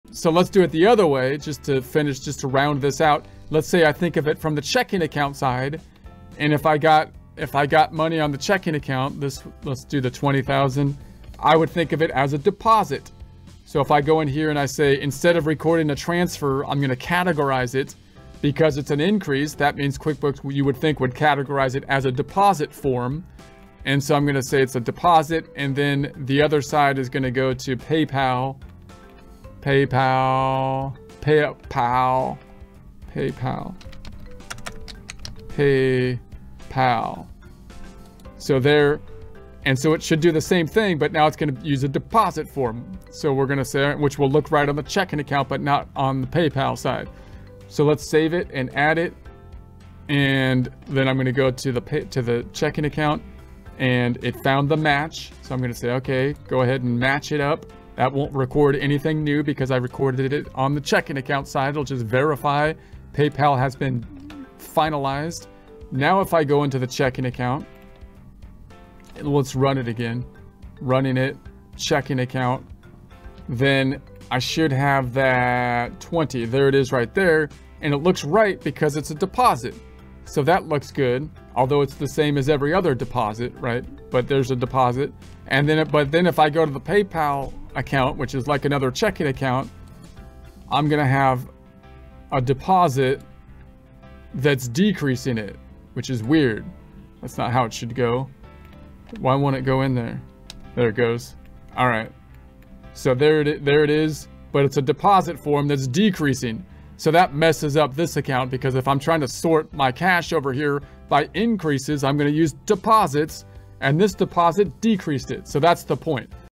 Bank Feed Transfer From PayPal Account to Checking Account Using Deposit Form - QuickBooks Online
Bank Feed Transfer From PayPal Account to Checking Account Using Deposit Form Comparing to Use of Transfer Form